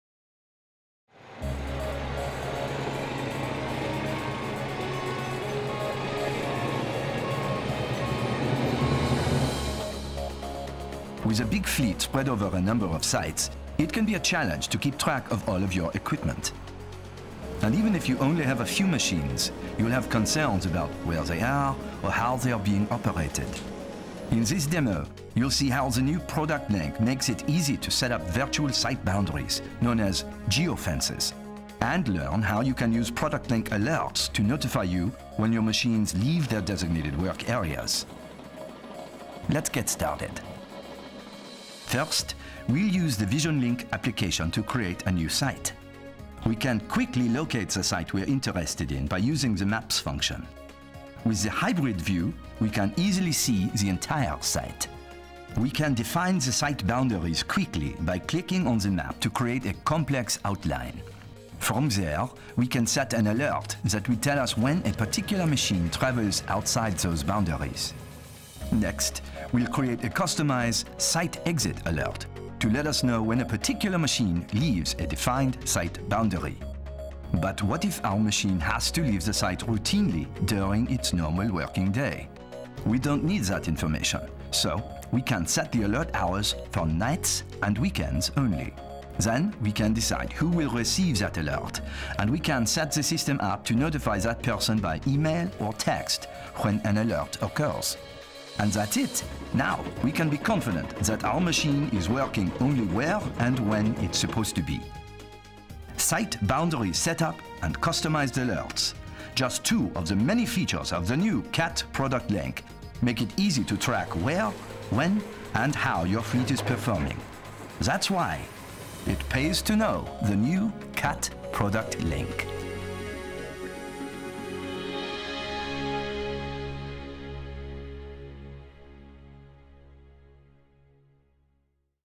Cat® Product Link™ | Site Boundaries | Geo-Fences (Europe)
With a big fleet spread over a number of job sites, it can be a challenge to keep track of what equipment is on what site. Cat® Product Link™ makes it easy it is to track the location of your equipment, set up virtual site boundaries (or, geofences) and improve the security of your assets.